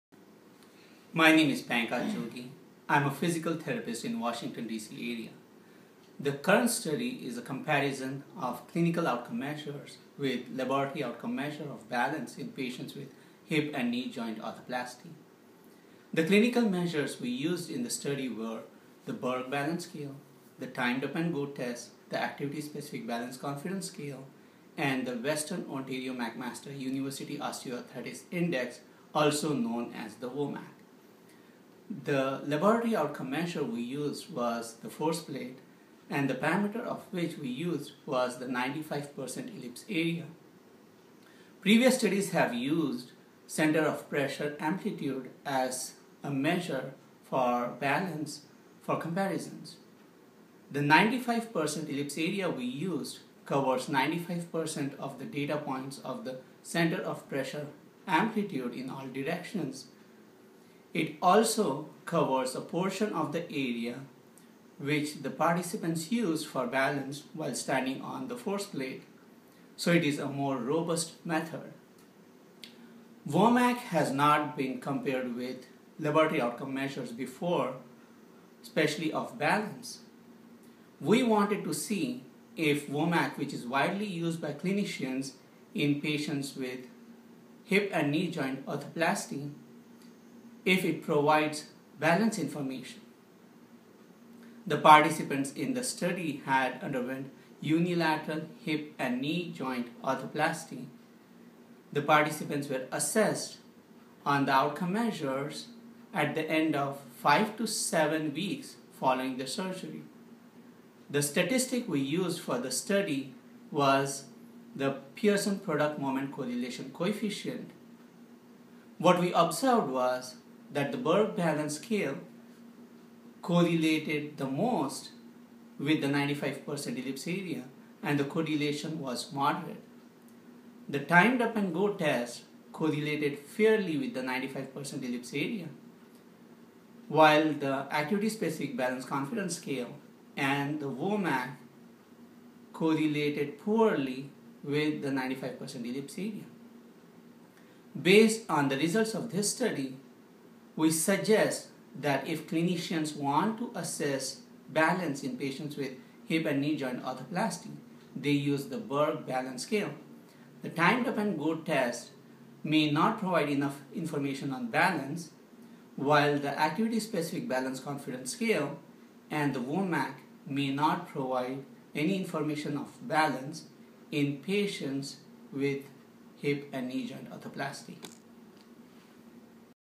Comparison of clinical and laboratory measures - Video Abstract 125581
Video abstract of Original Research paper “Comparisons of clinically based outcome measures and laboratory-based outcome measure for balance in patients following total hip and knee arthroplasty” published in the open access journal Orthopedic Research and Reviews by authors Jogi P, Overend T, Kramer J.

Background: Information available in the literature on clinically based and laboratory-based
outcome measures of balance is limited. How much information is provided by clinically based
outcome measures compared to laboratory-based measure in patients with total hip (THA) and
knee arthroplasty (TKA) is not known.
Aim: The purpose of this study was to determine the relationship between selected clinically
based outcome measures and laboratory-based force platform measure in patients following
THA and TKA.
Methods: Patients who underwent THA (n = 26) and TKA (n = 28) were evaluated at about
5–7 weeks following surgery. Participants were assessed using four clinically based outcome
measures – 1) the Berg Balance Scale (BBS), 2) the Timed Up and Go test (TUG), 3) the
Activities-specific Balance Confidence Scale (ABC), and 4) the Western Ontario McMaster
Universities Osteoarthritis Index-function subscale (WOMAC-function) – and one laboratorybased
force plate measure (95% ellipse area).
Results: Moderate correlations were observed between the BBS and the 95% ellipse area of
force plate (r = 0.46–0.51) for the two-legged stance, the anterior lean stance, and the posterior
lean stance. Fair correlations were observed between TUG and the 95% ellipse area of force
plate (r = 0.31–0.37) for all the three test conditions. Low correlations were observed for the
ABC and the WOMAC-function with the 95% ellipse area of force plate (r = 0.11–0.25) for
all the three test conditions.
Conclusion: The BBS demonstrated the greatest correlations with the 95% ellipse area of the
force plate measure and should be preferred by physical therapists over the TUG, the ABC, and
the WOMAC-function to assess balance in patients with THA and TKA.